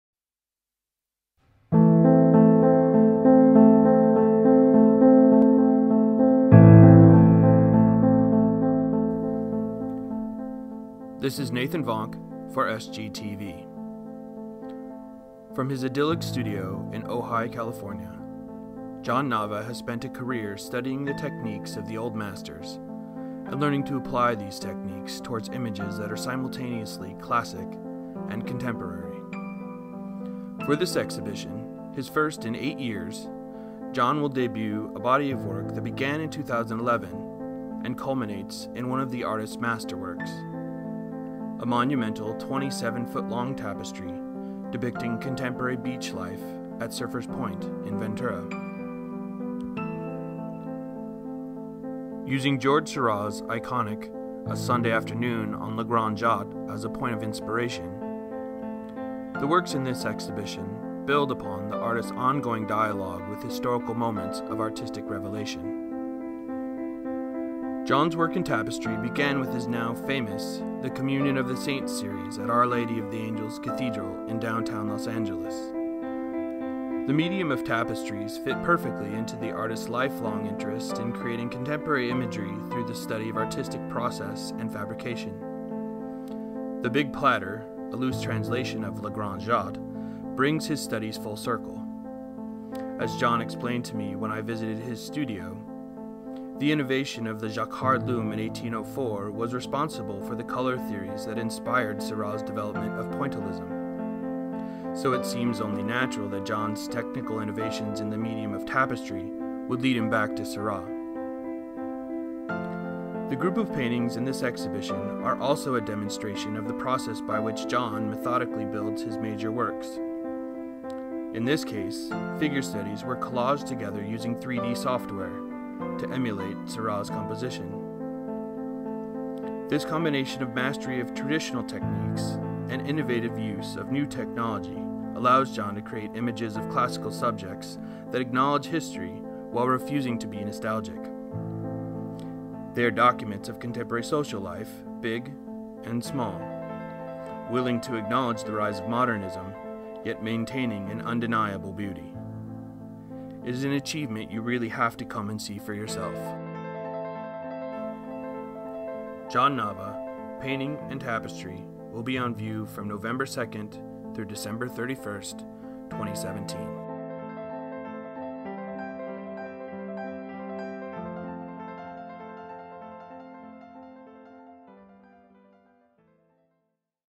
SGTV presents JOHN NAVA: Painting & Tapestry
In his first new exhibition in eight years, nationally acclaimed Ojai Realist John Nava presents a new heroically scaled tapestry as well as a suite of works created in connection with it.